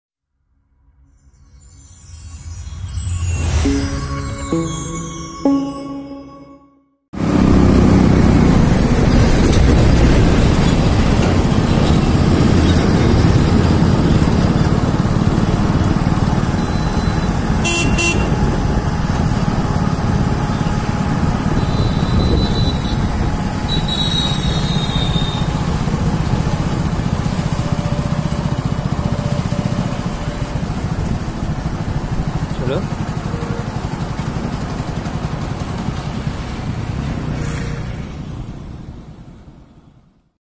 Unbelievable !! WAG9 on Road || Goods Locomotive of Indian Railways WAG9 on Road
I was passing throught NH-2 (Delhi - Kolkata) and suddenly I saw something like that i had never seen before, that was WAG9 is on National Highway, I was totally surprised, watch the full video and enjoy.

Device: - Redmi

Thankyou.